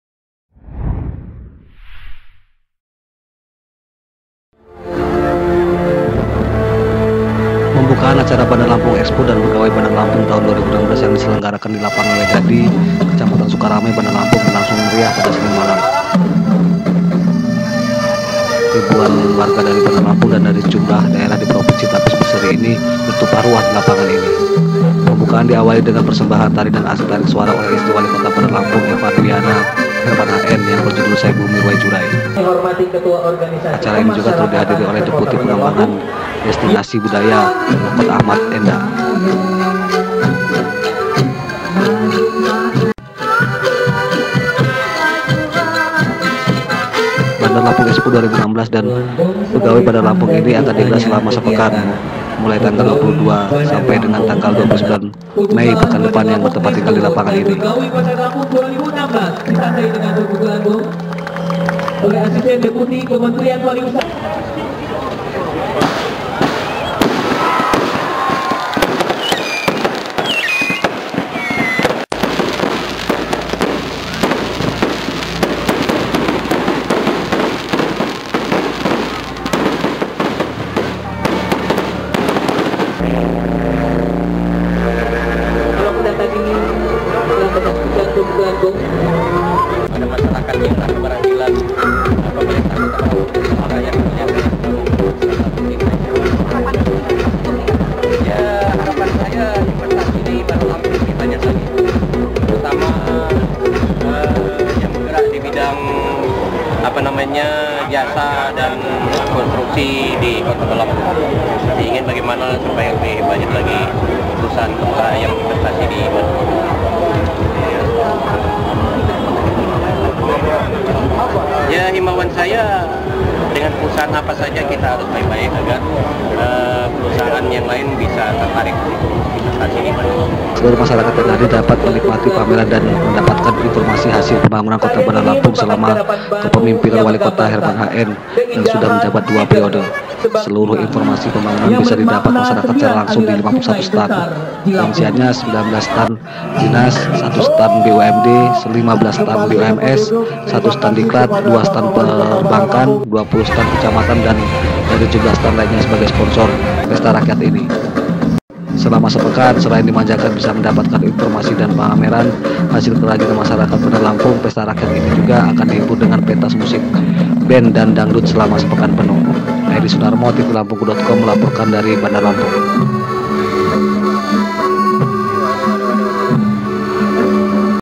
Ribuan warga hadiri Pembukaan Bandar Lampung Expo 2016
Pembukaan Bandar Lampung Expo dan Bandar Lampung begawi 2016 di Lampangan Way dadi, Sukarame, Bandar Lampung berlangsung meriah. Ribuah warga hadiri acara pameran pembangunan dan pesta rakyat ini. ( edy sunarmo)